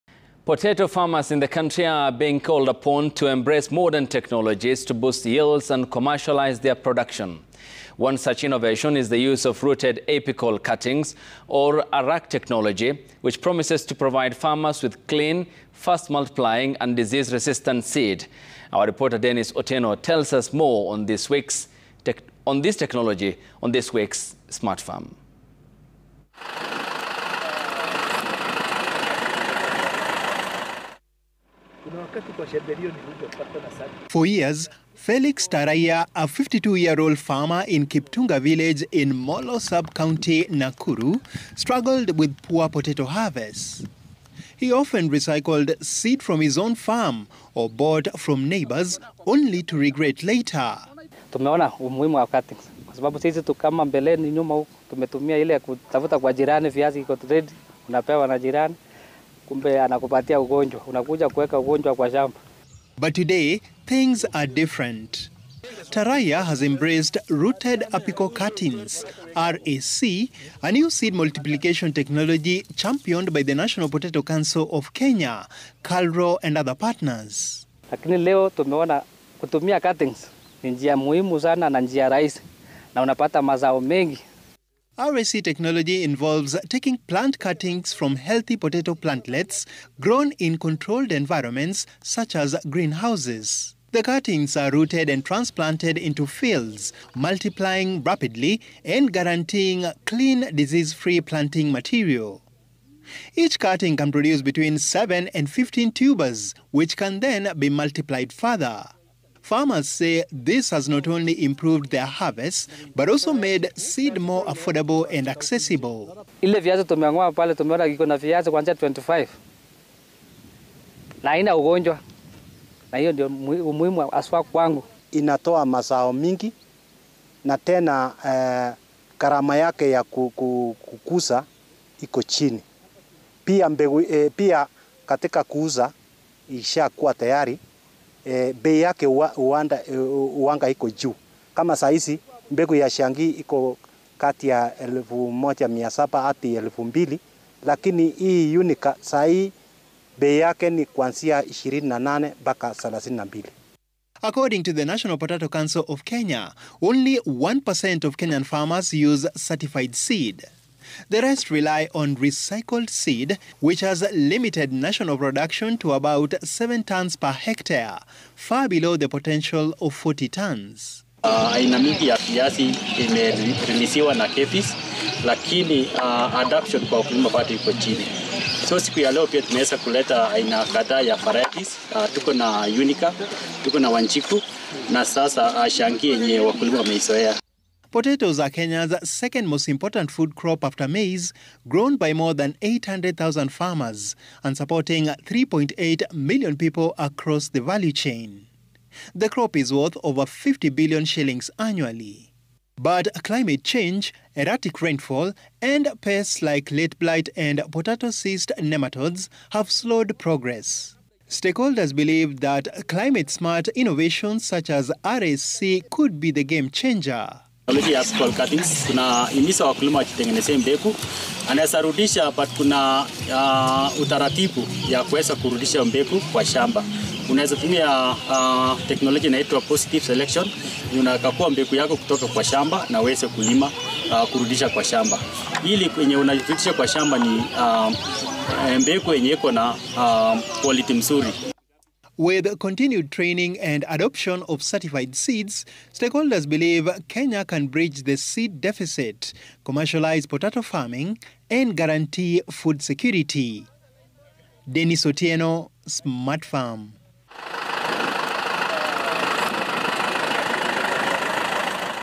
Smart Farm: Nakuru farmers adopt apical cutting tech to boost potato yields
Potato farmers in the country are being urged to embrace modern technologies to boost yields and commercialize their production. One such innovation is the use of Rooted Apical Cuttings, or RAC technology, which promises to provide farmers with clean, fast-multiplying, and disease-resistant seed.